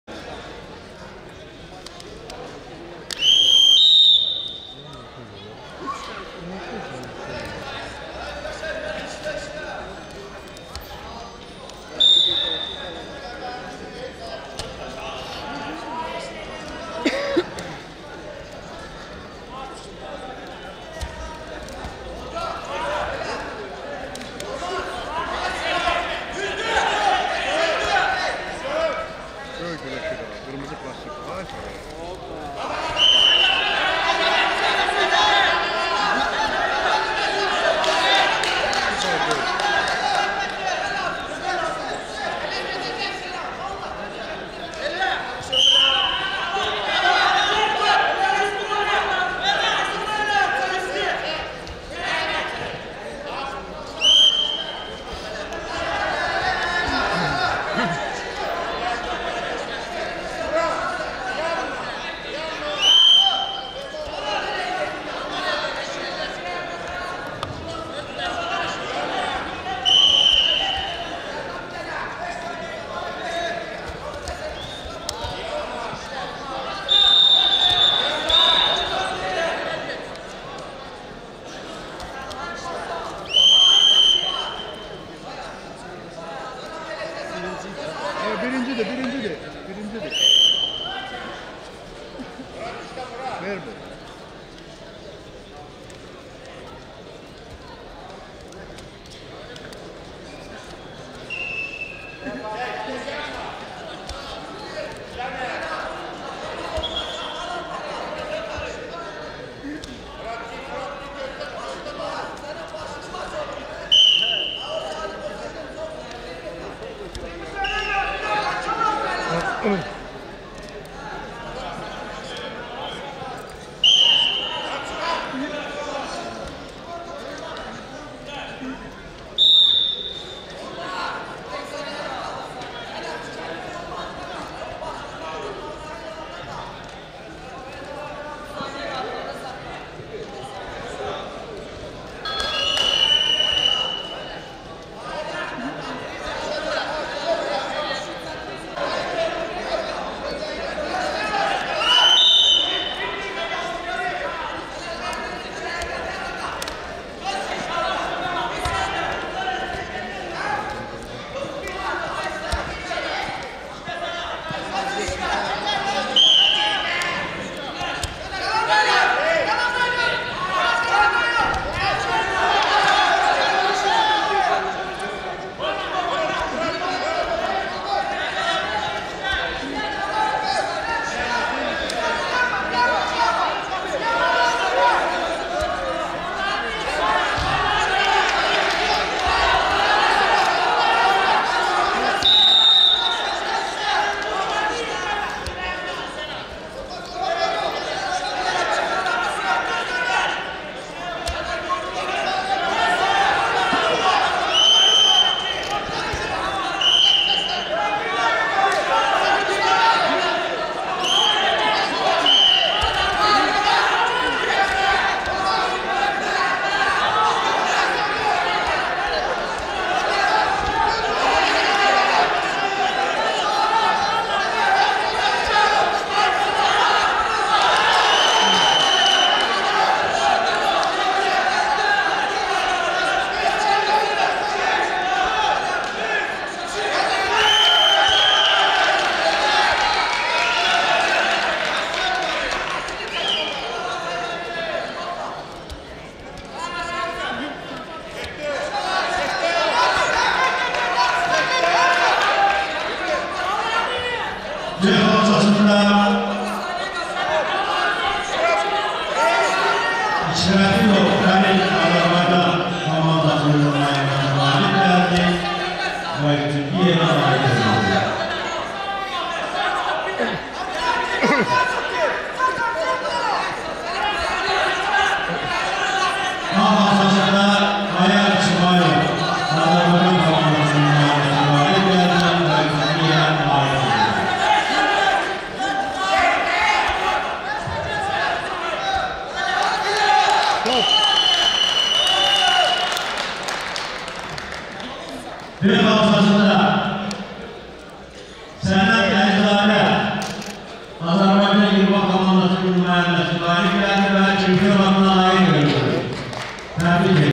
Müşviq İsayevin xatirə turniri - sərbəst - 85 kq final
Müşviq İsayevin xatirə turniri - sərbəst güləş -  85 kq final / 1. Sənan Əliyev  2. Murad Həsənov